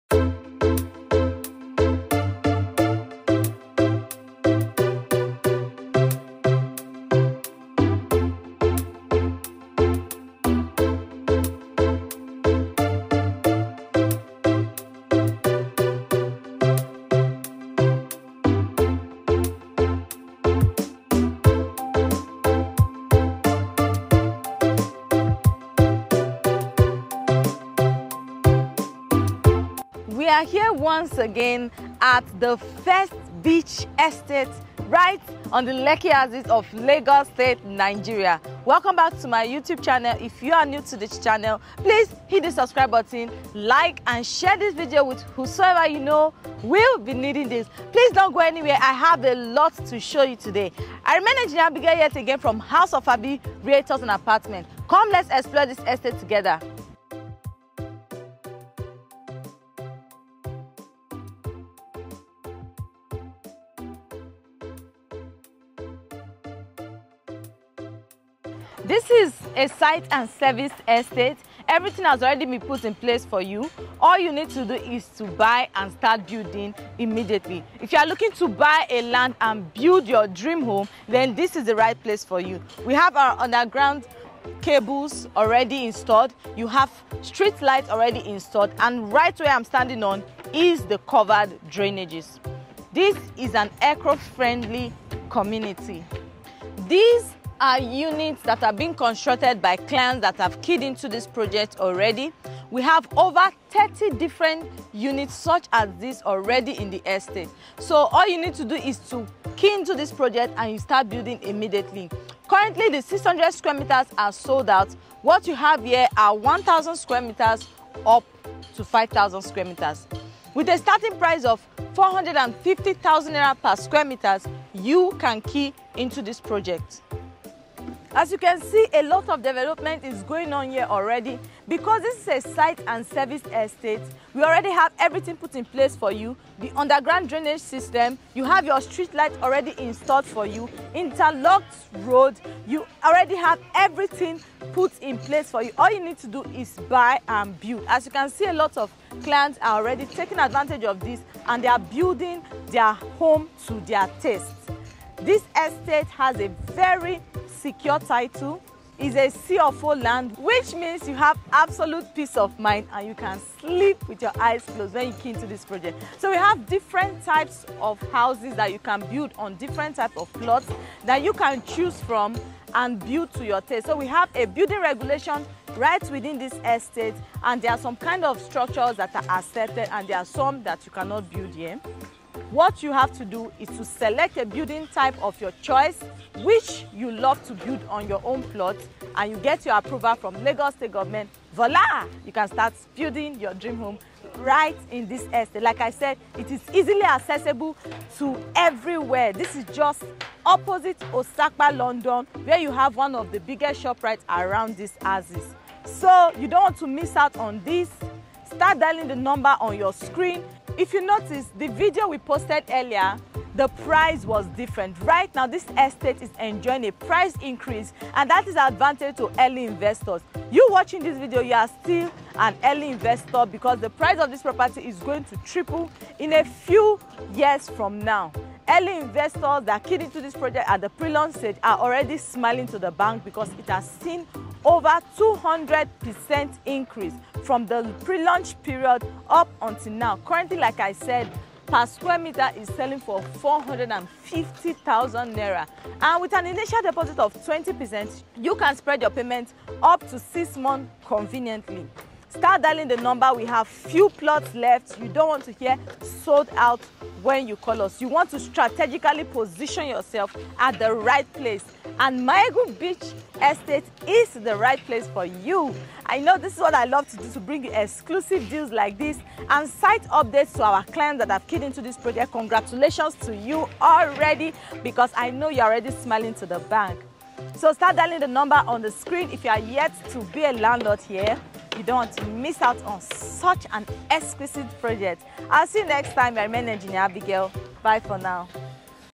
Inside Lekki's first beachfront estate. Build your own home here
WELCOME TO THE FIRST BEACH FRONT ESTATE IN LEKKI.

SPANNING OVER 50 hectares OF LAND.

THIS FULLY SERVICED ESTATE HAS LUXURY PLOTS SELLING, FOR INVESTORS WITH GREAT VISIONARY.

FROM LAUNCHING TILL DATE, EARLY INVESTORS HAVE MADE OVER 100% CAPITAL PROFITS AND MASSIVE RETURNS.
THIS INVESTMENT IS ONE THAT WILL DEFINITELY BOOST YOUR PORTFOLIO.

Luxury is peace. Luxury is serenity

With over 1km of private beach front and a wide promenade with sweeping views of Victoria Island and Eko Atlantic in the distance,

Maiyegun Beach Estate is naturally serene, and its cool winds and sandy beaches will make sure you always wake up with a smile; refreshed and ready to face each new day.

LOCATION:JAKANDE LEKKI

Price starting from #450,000 per sqms
( limited plots available)

Facilities
Underground drainage
Underground electrical cables
Central sewage treatment
Fiber optic internet
Uninterrupted electricity
Games arena
Recreational center
Pool
Gym
Eco Friendly system

Give us a call/WhatsApp for inspections and purchase